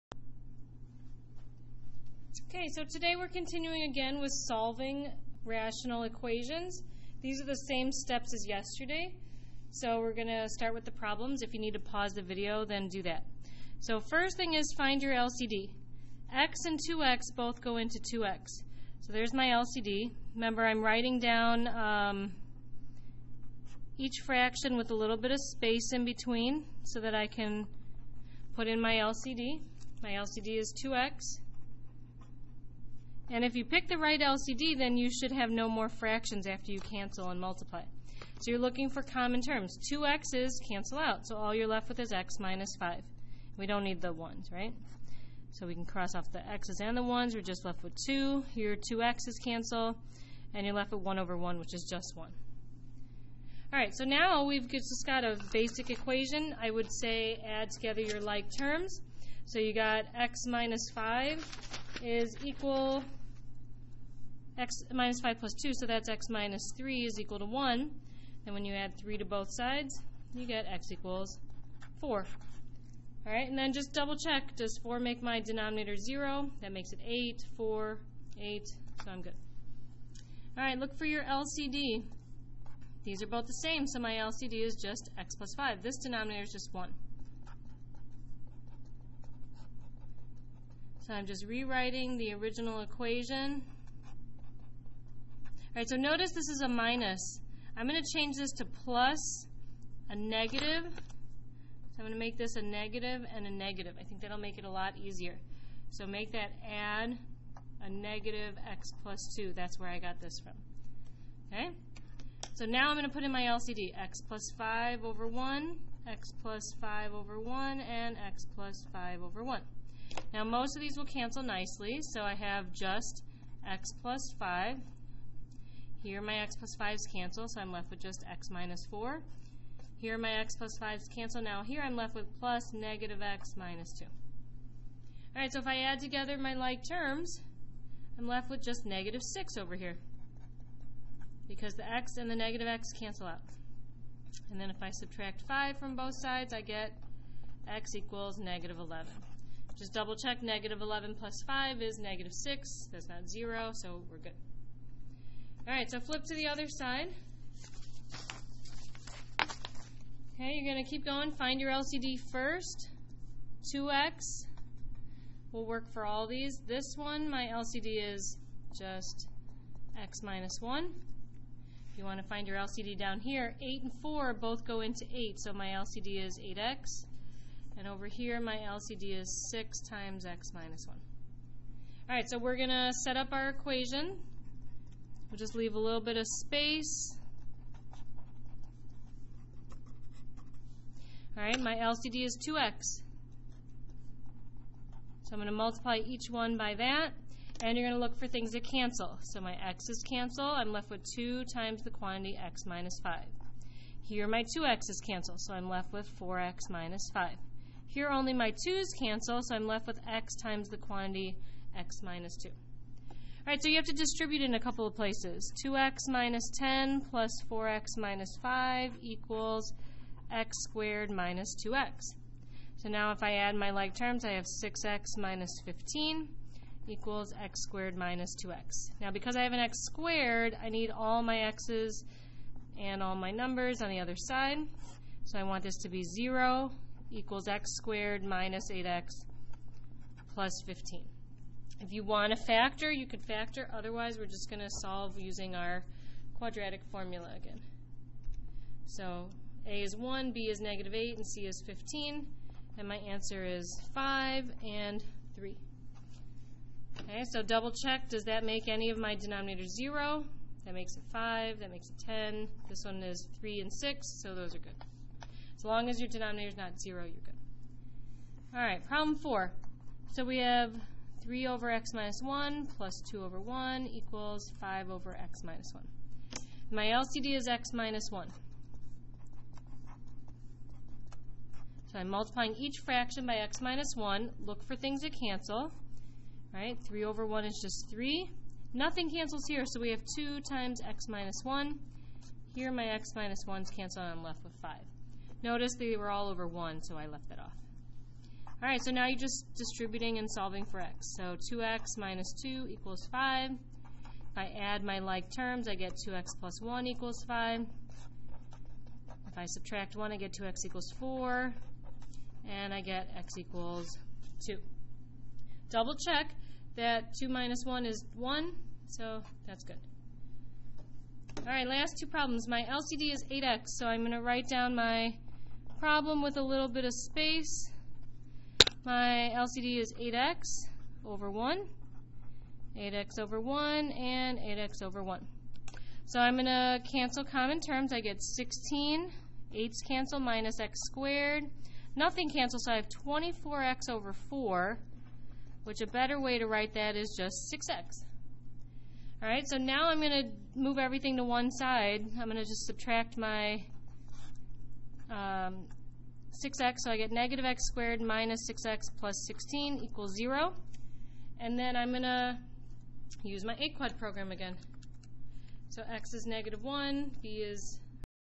Unit 10 Day 10 Video Notes
Solving Rational Equations (we will do #6 in class together)